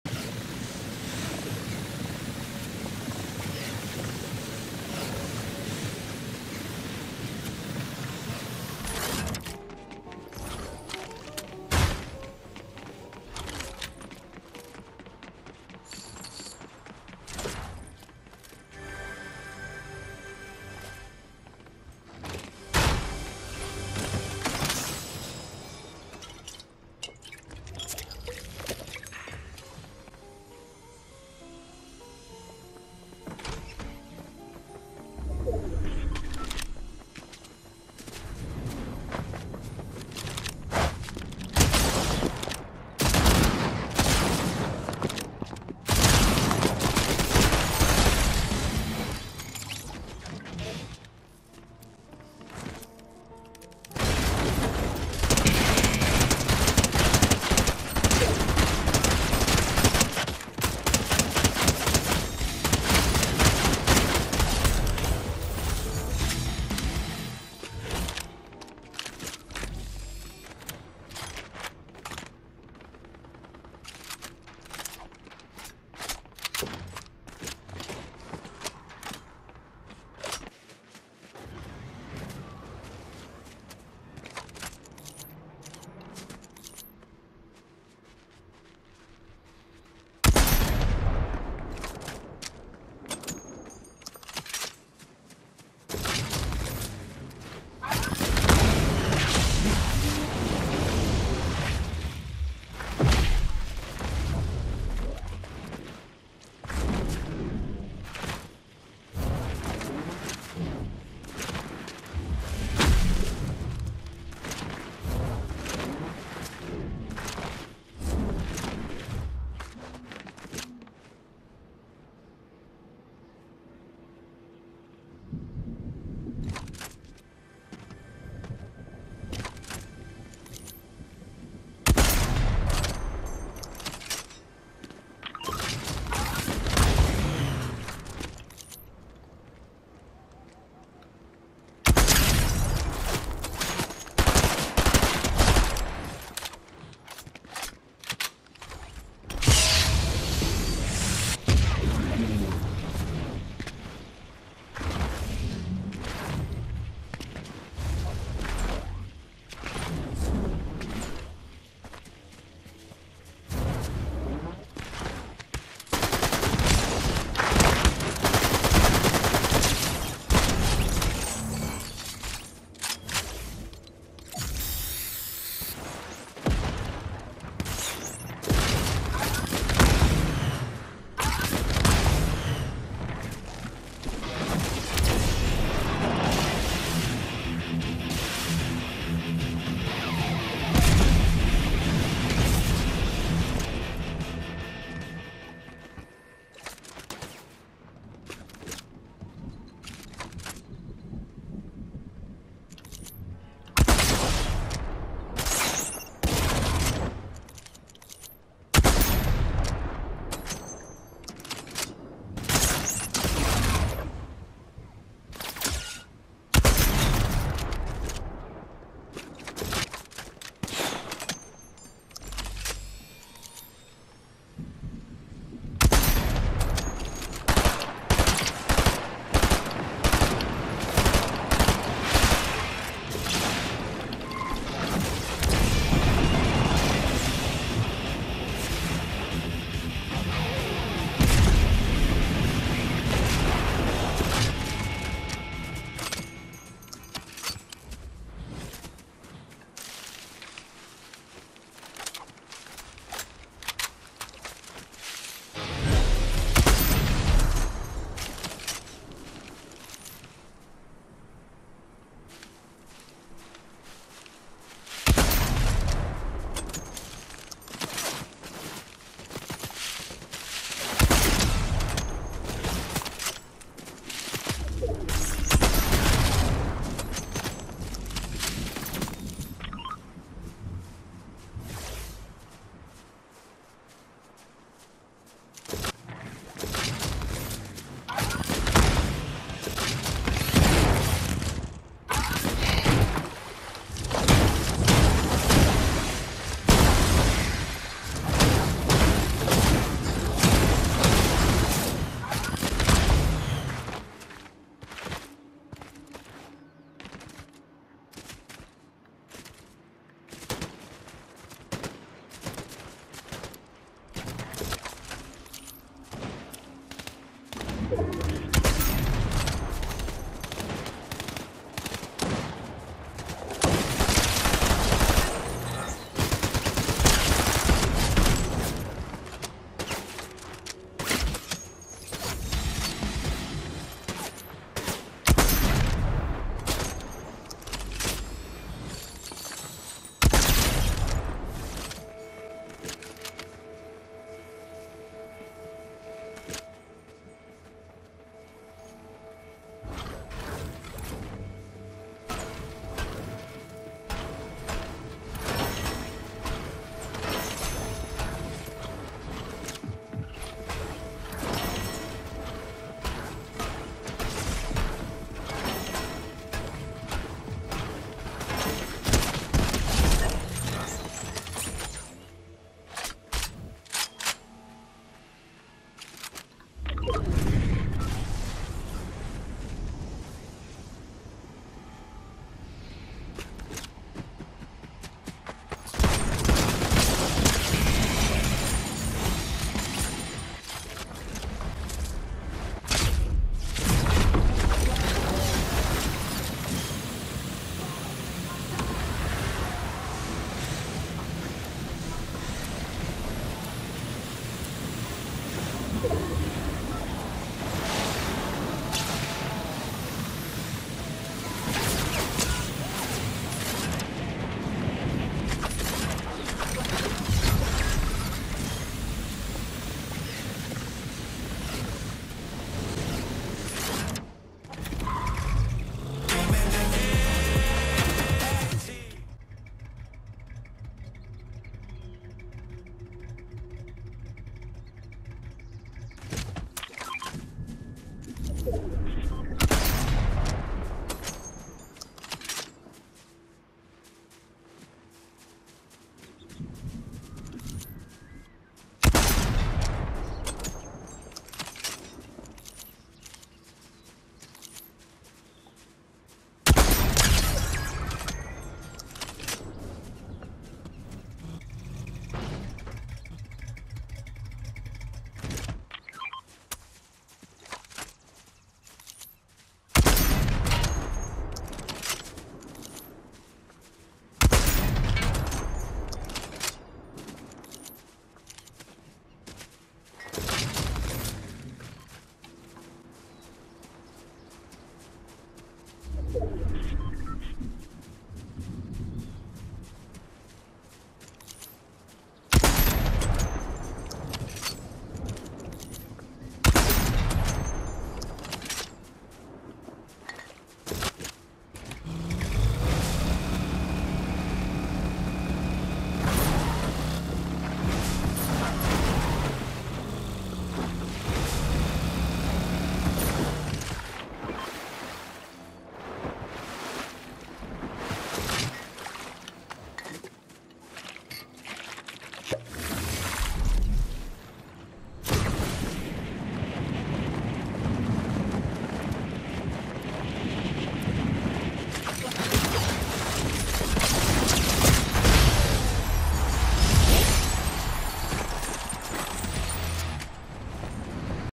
Solo W for the Day. Pt 1
After a long day at work. Another Victory Royal for the OGs june 26th 2024